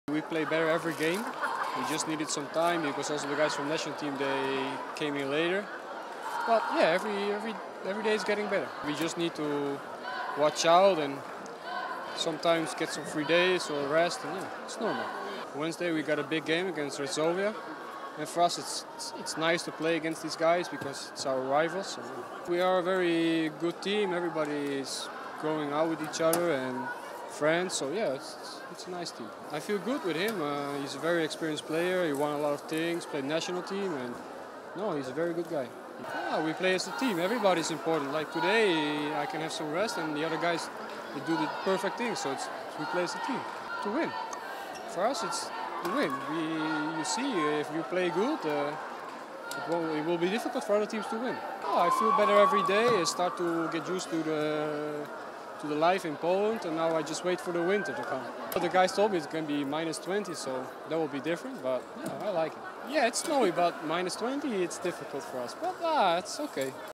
Dick Kooy (ZAKSA): Czekam na zimę i mecz z Resovią
Dick Kooy, przyjmujący ZAKSY Kędzierzyn-Koźle, przyzwyczaja się do życia w Polsce,. Holender jest zadowolony z atmosfery w drużynie i uważa, że jego zespół jest coraz silniejszy. A na co czeka i jak gra mu się z Pawłem Zagumnym?